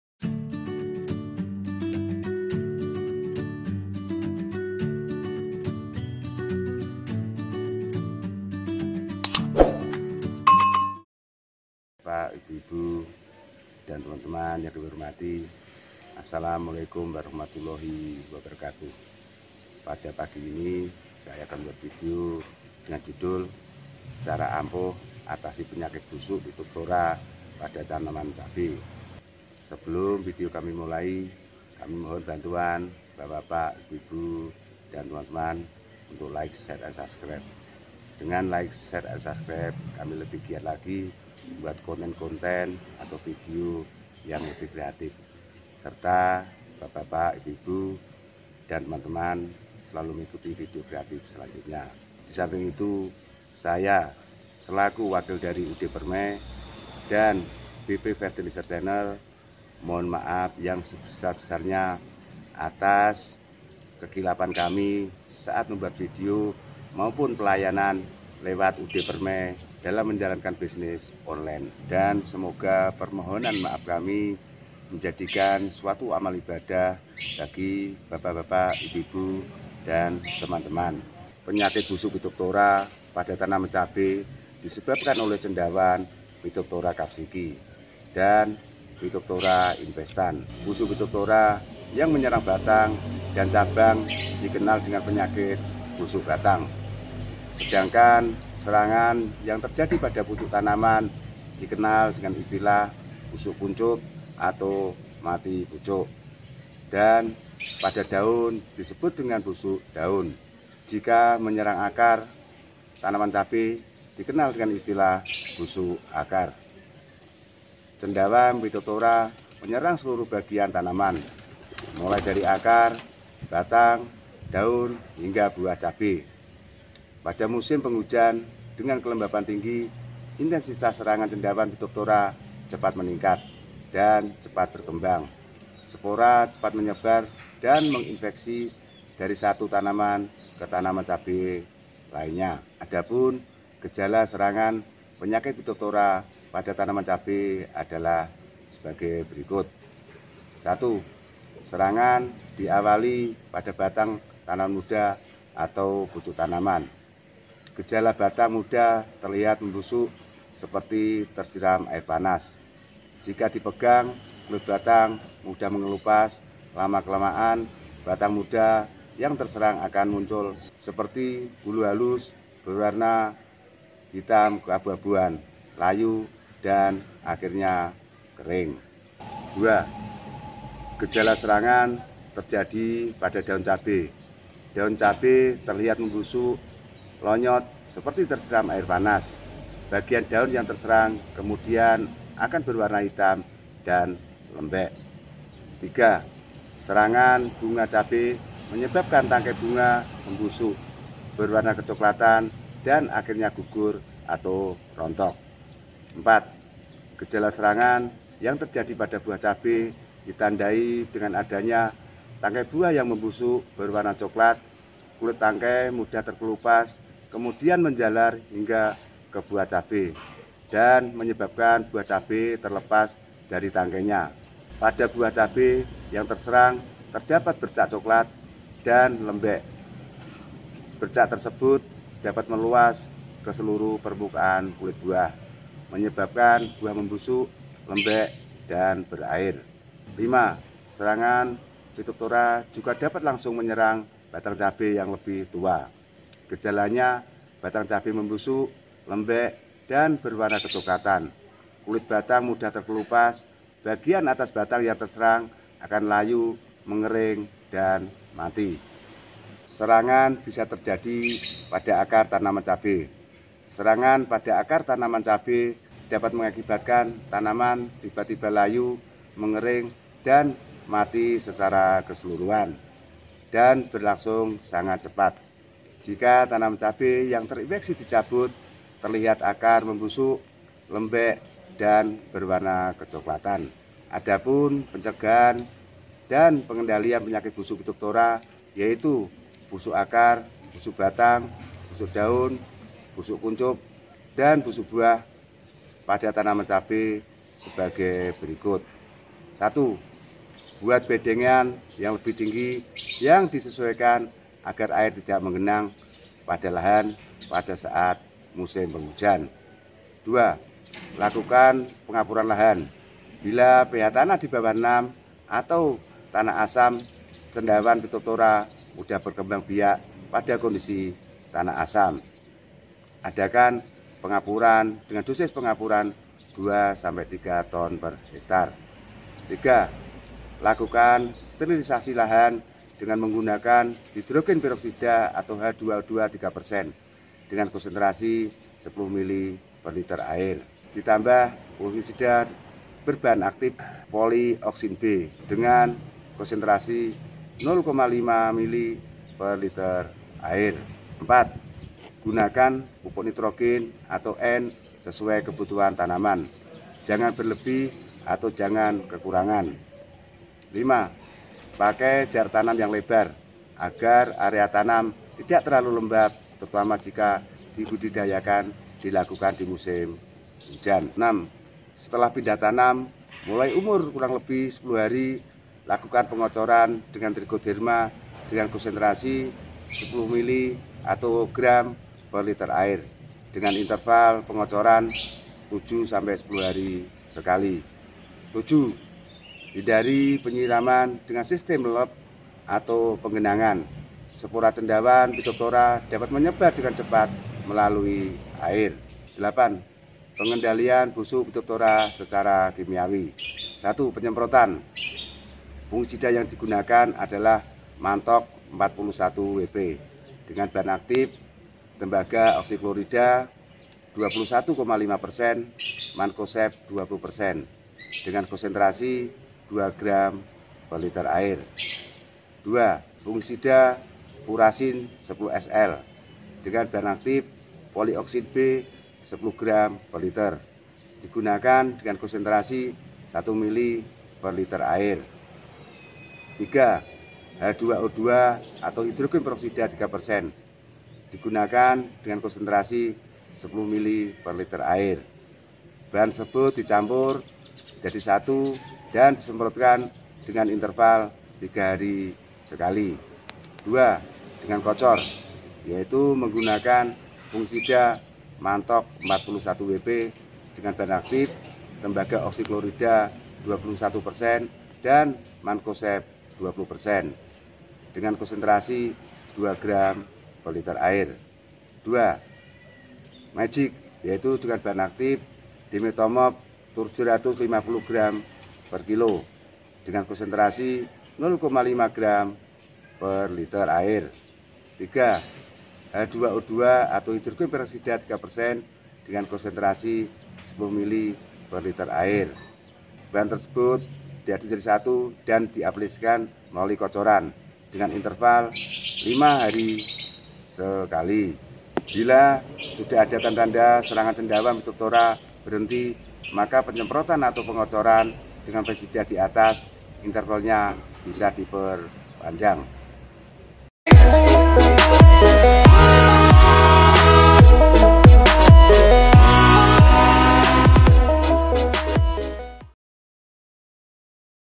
CARA AMPUH ATASI PENYAKIT BUSUK PHYTOPHTHORA PADA TANAMAN CABE
MEMOTIVASI PETANI DALAM MENGATASI  PENYAKIT BUSUK PHYTOPHTHORA PADA TANAMAN CABE